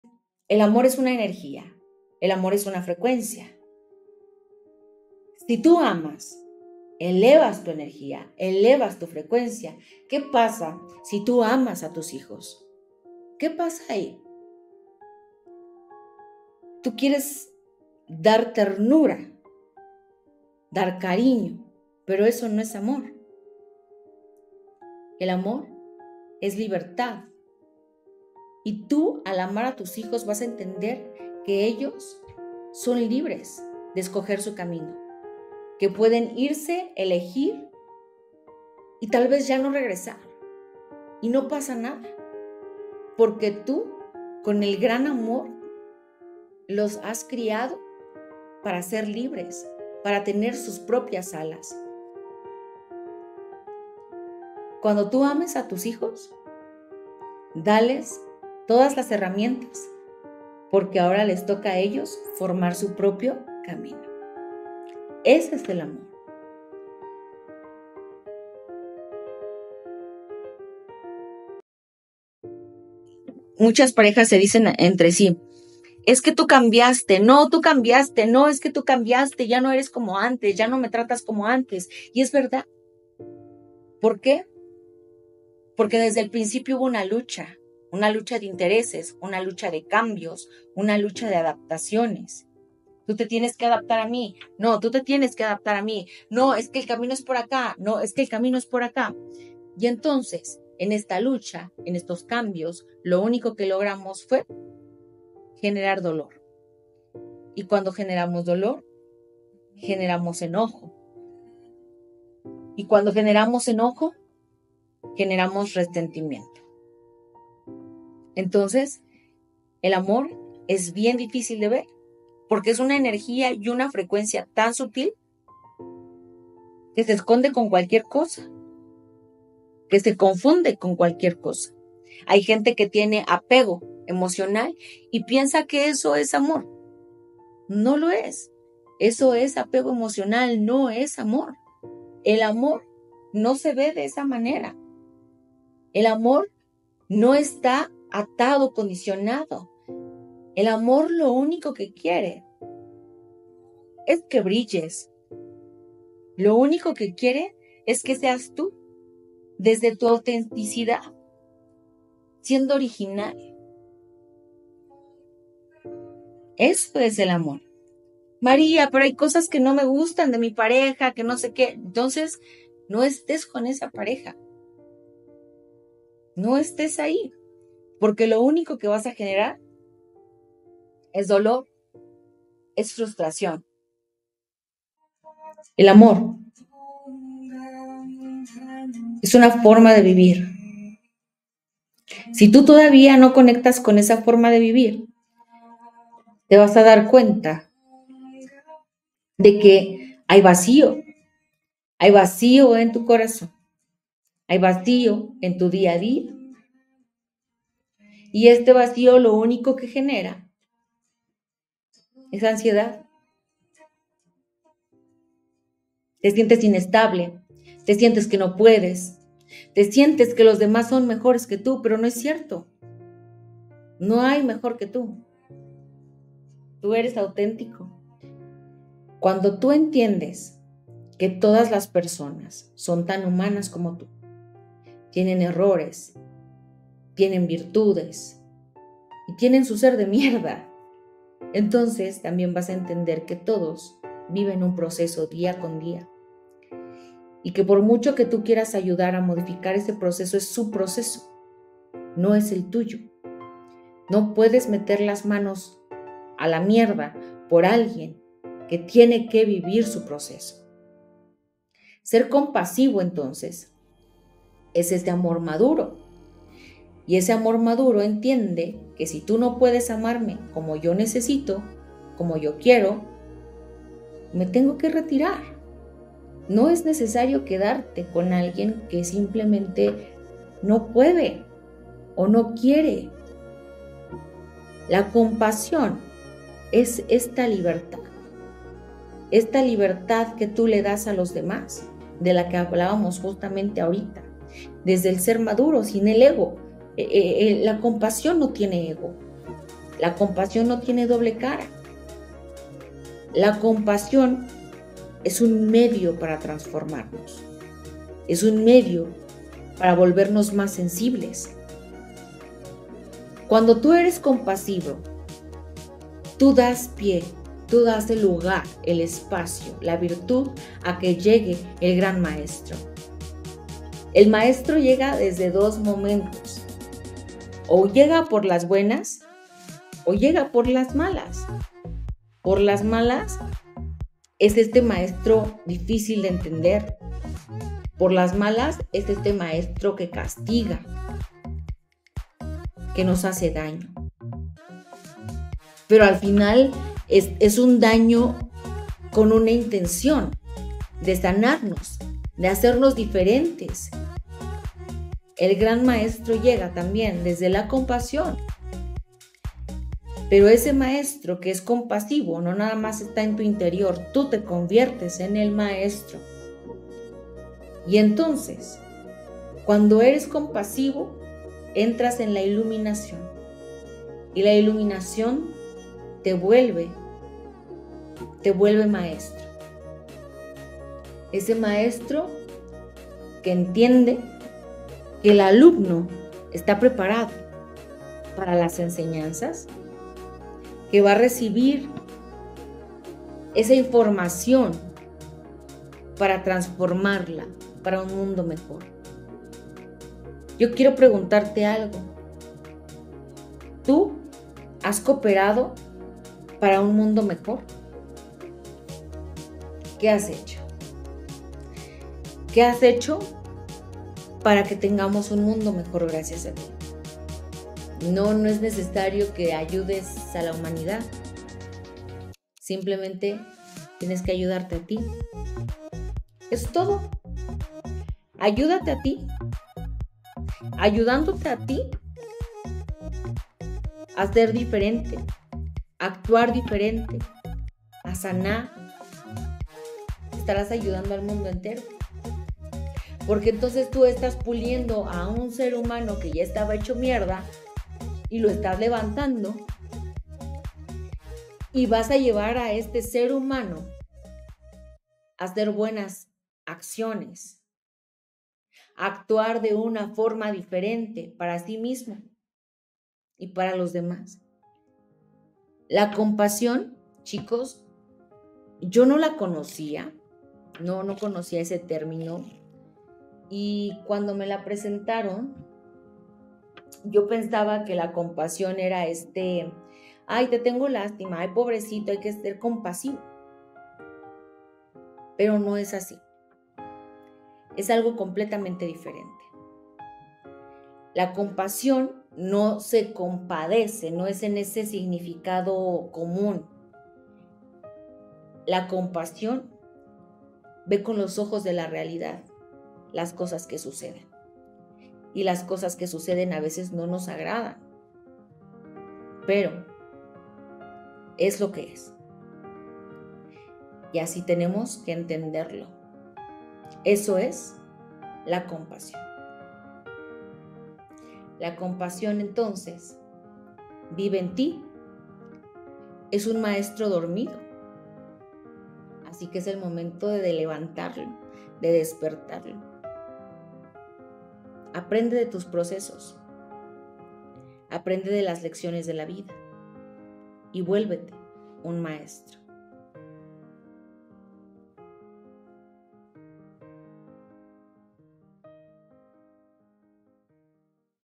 El amor y la compasión
Te invito a ver este clip sobre el amor y la compasión, breves ejemplos de la vida cotidiana, cual sería tu duda?